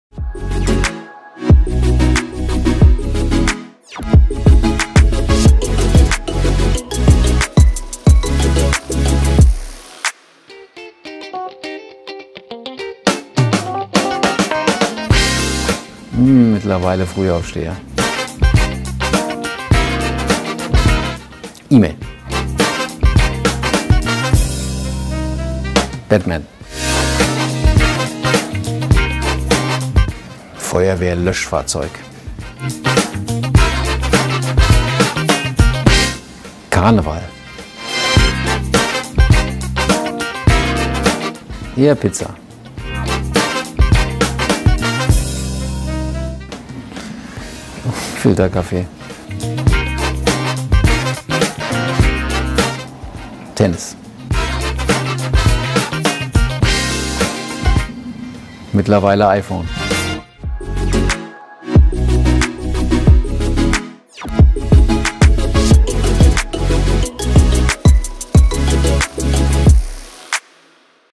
Halbzeitbilanz 2021: THIS or THAT mit Innenminister Peter Beuth
Fußball ⚽ oder Tennis? Polizeiwagen oder Feuerwehrlöschfahrzeug 🚒? Die Antworten auf diese und weitere Fragen gibt Innenminister Peter Beuth in einer neuen Folge unserer Serie THIS or THAT. Wie hättet Ihr Euch entschieden? :-)

➡️ P. S. Weitere Informationen zur Halbzeitbilanz sowie einen Ausblick auf alle relevanten landespolitischen Themen gibt es